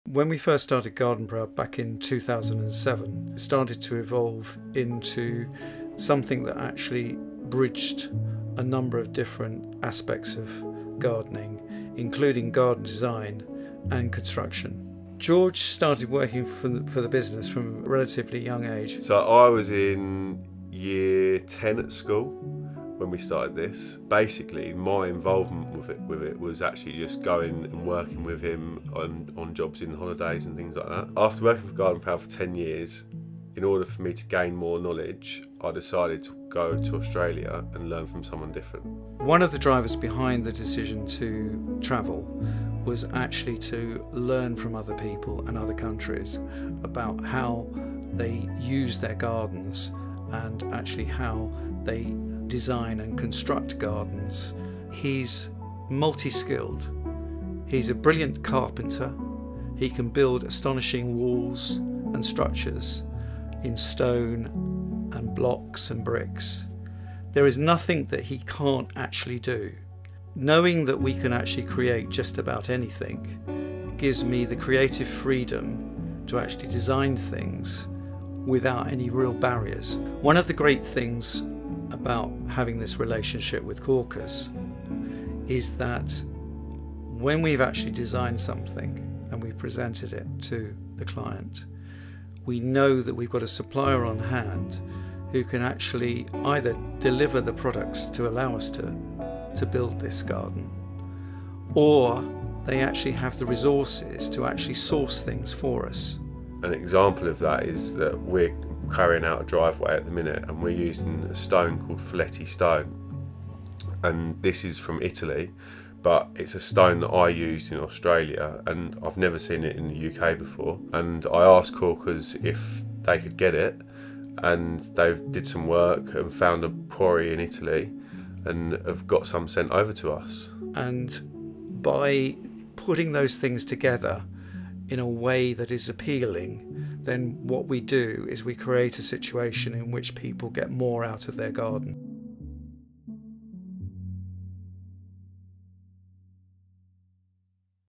Meet Our Show Garden Partner | Garden Proud
We are delighted to introduce you to one of our esteemed partners, Gardenproud, based in the charming town of Tunbridge Wells, Kent. This family-run company has been a cherished collaborator of ours for over a decade, and we have had the privilege of witnessing their remarkable growth and success over the years.

Gardenproud is a visionary organization that holds a deep appreciation for both architecture and plants. They possess a holistic approach to garden design, landscaping, and maintenance, which is reflected in their outstanding work.

Founded by Tim Sykes in 2007, Gardenproud has flourished and expanded through its own teams and partners. Today, they offer innovative landscaping solutions across Kent, East Sussex, and Hertfordshire.

Their design and construction projects range from £5,000 to £250,000, and they bring an unparalleled level of expertise and care to every project they undertake.

If you are seeking the perfect team to breathe new life into your garden this year, we strongly recommend getting in touch with Gardenproud before the summer season. They are sure to exceed your expectations and create a breath-taking outdoor oasis for you to enjoy.